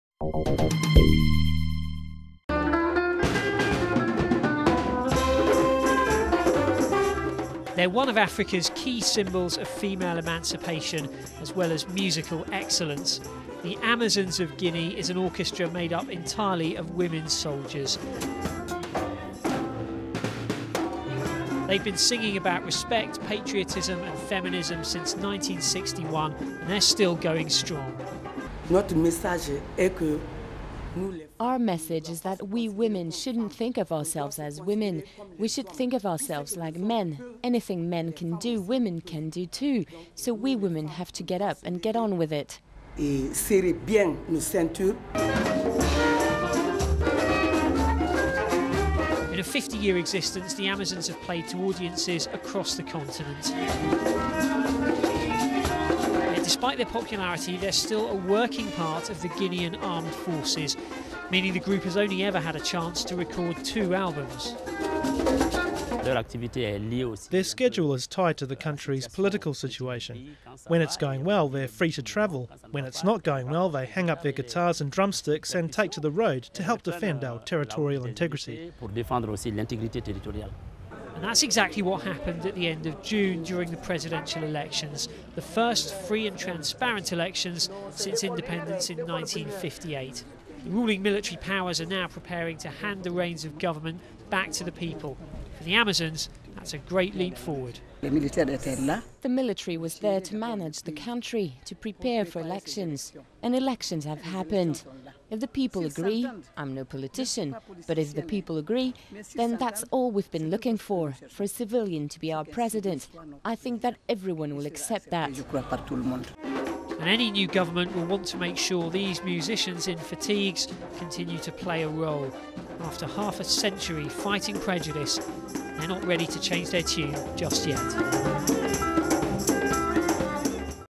Women, soldiers, musicians -- Guinea's Amazonian orchestra
Marching to their own drum -- the women of the famous Amazons of Guinea orchestra have been doing just that for half a century. The female-only band are all members of Guinea's armed forces -- and when they're not fighting to secure the country's political future, they're fighting prejudice and discrimination with their music.Duration: 02:11